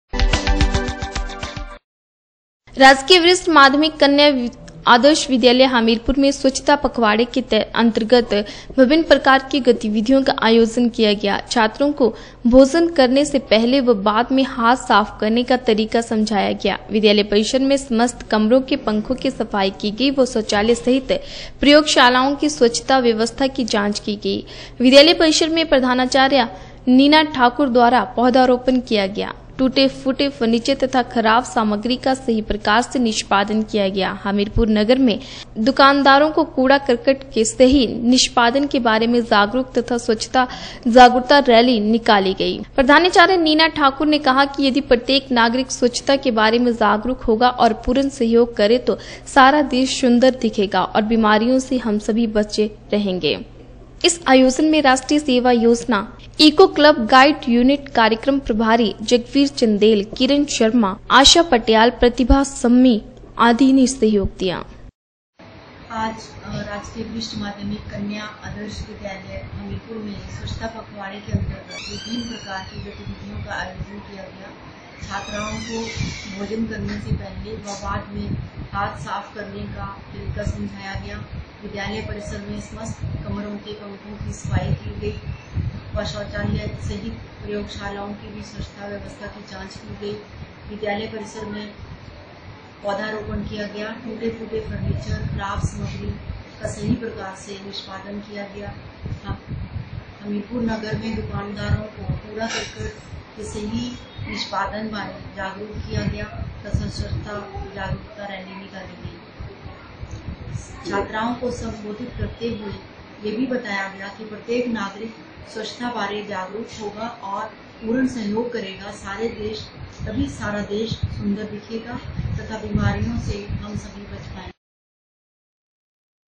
स्वच्छता पखवाडे के अन्र्तगत विभिन्न प्रकार के कार्यका्रमो का आयोजन किया गया
राजकीय वरिष्ठ माध्यमिक कन्या विद्याायल हमीरपुर में स्वच्छता पखवाडे के अन्र्तगत विभिन्न प्रकार के कार्यका्रमो का आयोजन किया गया व शहर में रैली निकाली गई ।